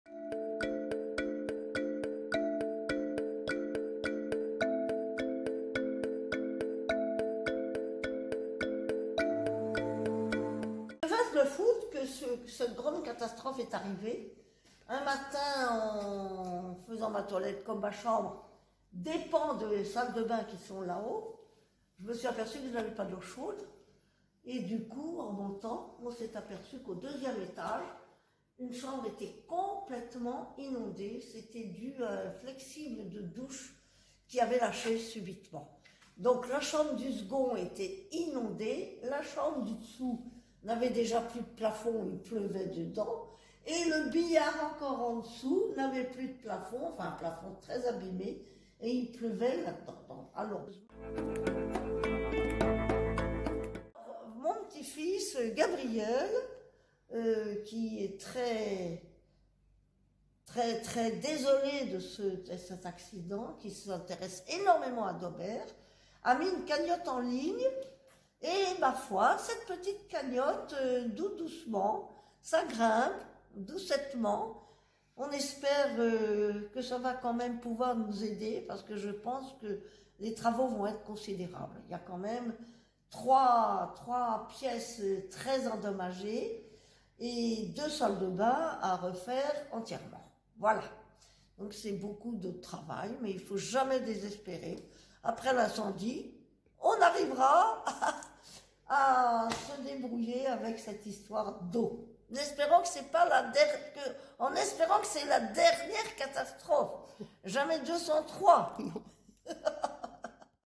Sarthe. Inondation au château de Dobert à Avoise : "l'eau on ne l'arrête pas"
Hélène du Peyroux, propriétaire du château de Dobert à Avoise (Sarthe) revient sur l'important dégât des eaux qui s'est produit le 29 août dernier.